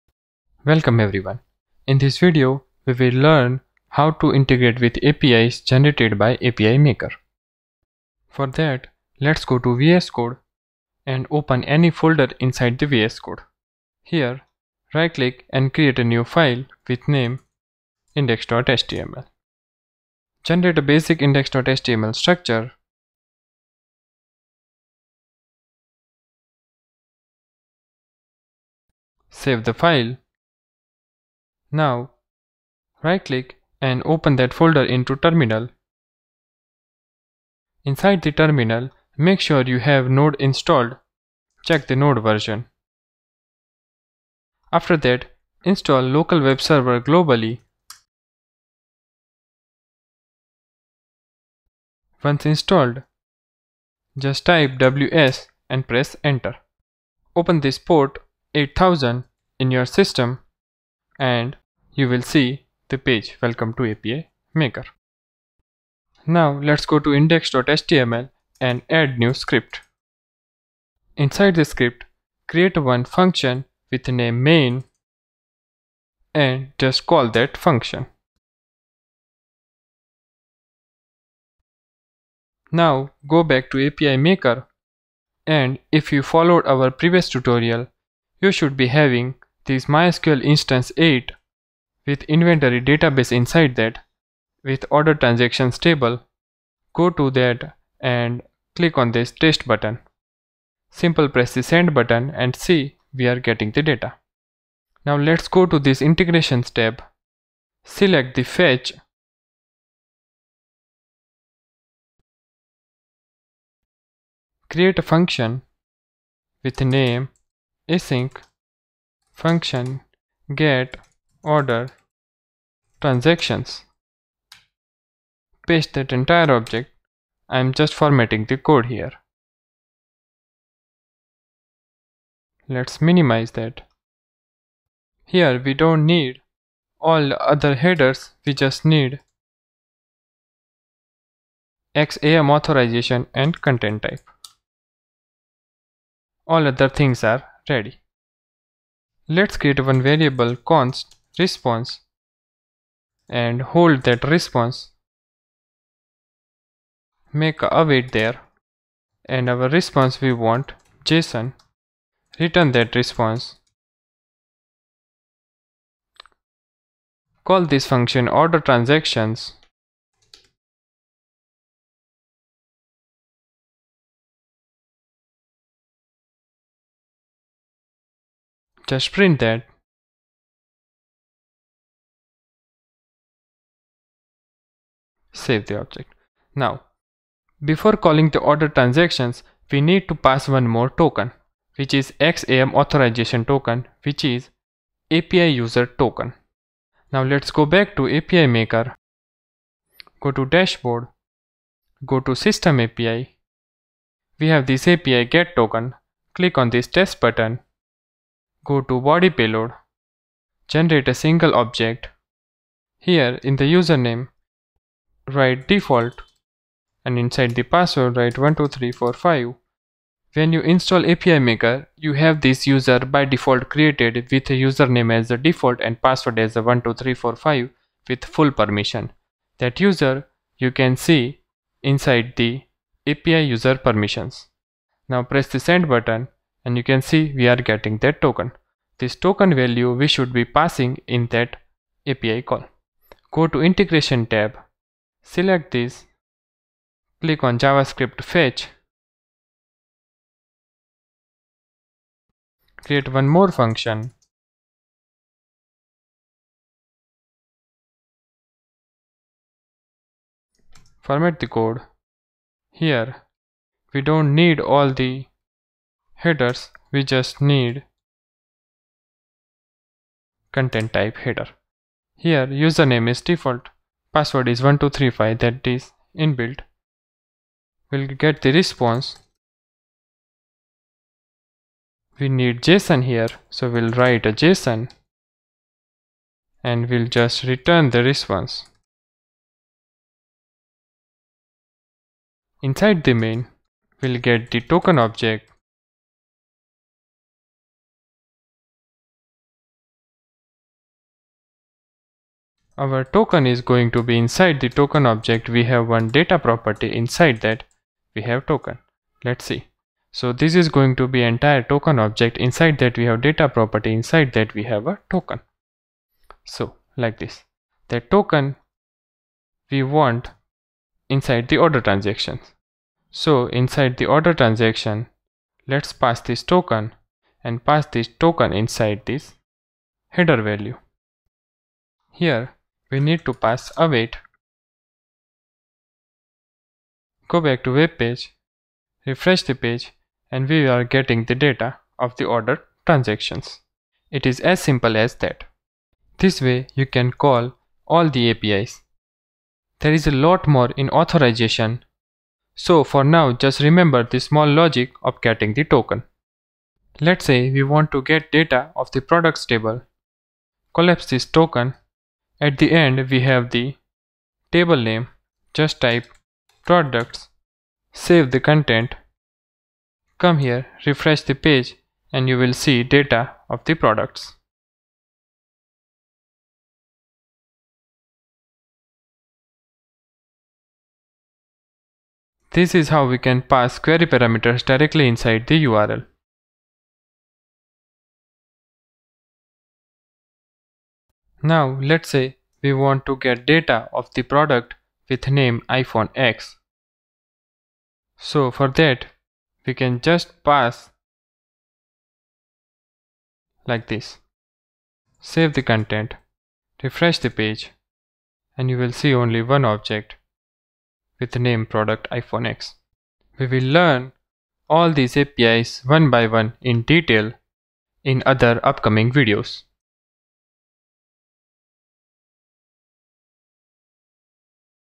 2 Using REST APIs & Generating APIs from MySQL, PostgreSQL & MongoDB Is Super Easy
🚀 Discover why generating & using APIs are super easy with API Maker—the most powerful backend framework for Node.js and JavaScript developers.

In this tutorial, we’ll show you how to integrate with APIs generated by API Maker, connect your database, and fetch data in just a few lines of code.

Whether you’re new to backend development or a seasoned engineer, you’ll see how API Maker makes the entire process smooth and frustration-free.

With API Maker, you can connect to MongoDB, MySQL, Microsoft SQL Server, MariaDB, Oracle Database, PostgreSQL, TiDB, and Percona XtraDB and instantly generate REST APIs without writing repetitive boilerplate.

Instead of spending hours setting up endpoints, authentication, and CRUD logic, API Maker gives you production-ready APIs out of the box—making backend development as simple as writing front-end JavaScript.

🔑 What you’ll learn in this video:
• How to set up a basic index.html file and integrate with APIs generated by API Maker.
• Using Node.js, VS Code, and a local web server to test API calls step by step.
• How API Maker automatically generates REST endpoints for databases like MySQL, PostgreSQL, SQL Server, MongoDB, Oracle, MariaDB, TiDB, and Percona XtraDB.
• The simple process of fetching data with JavaScript, handling tokens, and calling multiple APIs (orders, products, queries).
• Why API Maker is a powerful Supabase alternative and Appwrite alternative, designed to give developers more flexibility, scalability, and speed.

👉 If you’re building modern backends and want a faster way to generate APIs, this video is for you. Supabase and Appwrite provide great services, but API Maker goes beyond by giving you full control, self-hosting options, and seamless support for multiple databases. Instead of being locked into a platform, you own your APIs and can deploy them anywhere—on your own infrastructure or in the cloud.

📌 If this tutorial helps you, give it a thumbs up 👍 and share it with your dev community.

Try out API Maker and see how generating & using APIs are super easy when done right.